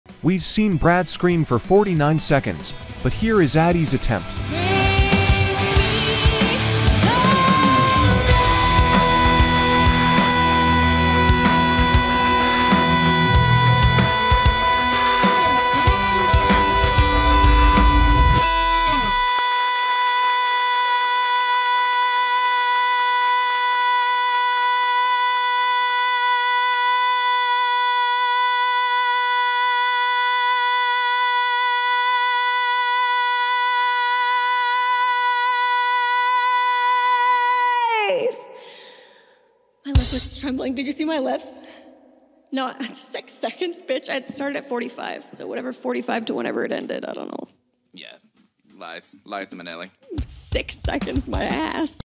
Addie attempts to hold a note for as long as possible
Next Stream Dates on TWITCH (usually): 🎵WEDNESDAY 4pm PT 🎵FRIDAY 1pm PT